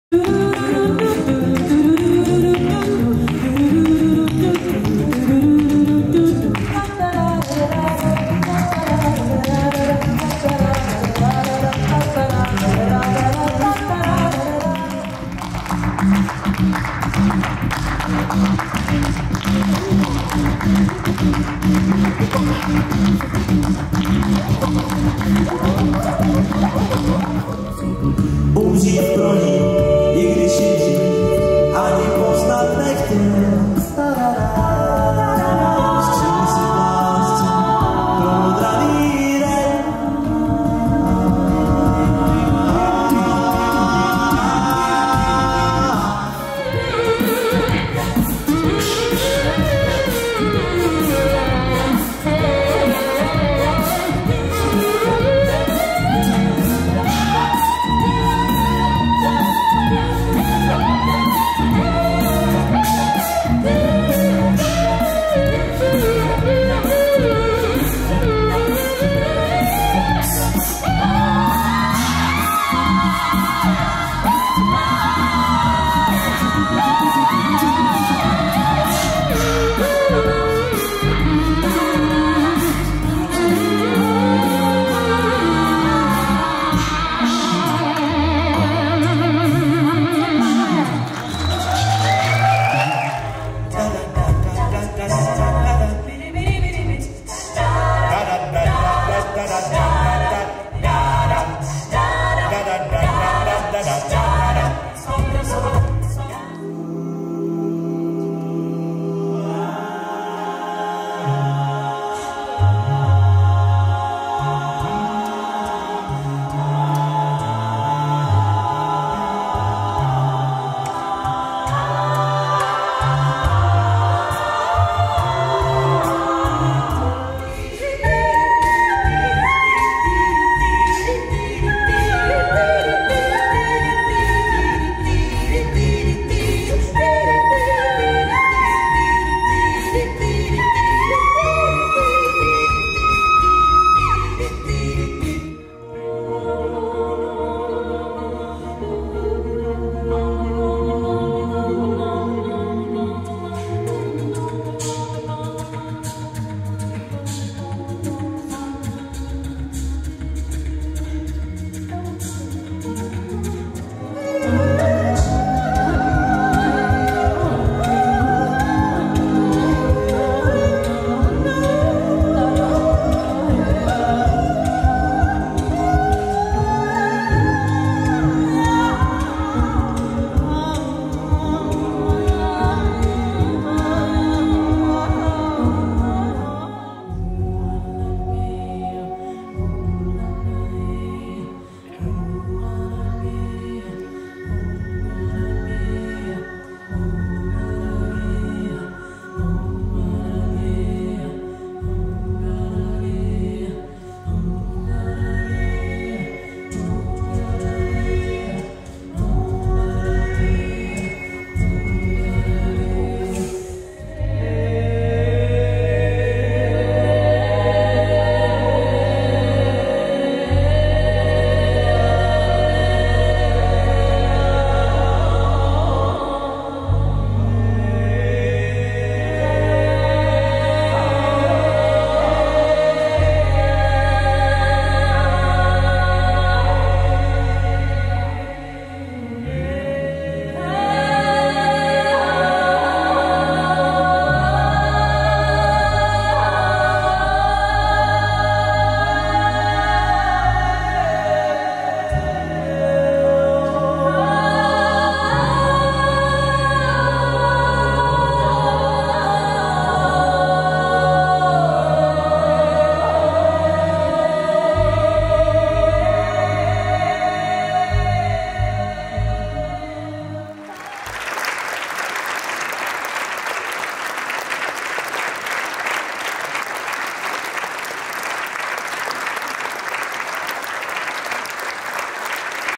SKETY: live in Prague Observatory with 8K full dome projection
Our concert in the Observatory was really extraordinary due to the "full dome" projections of cosmos in 8K resolution. We've had a great time improvising on what we were seeing all around us.

Video courtesy of Martin Festr Vít and Prague Observatory.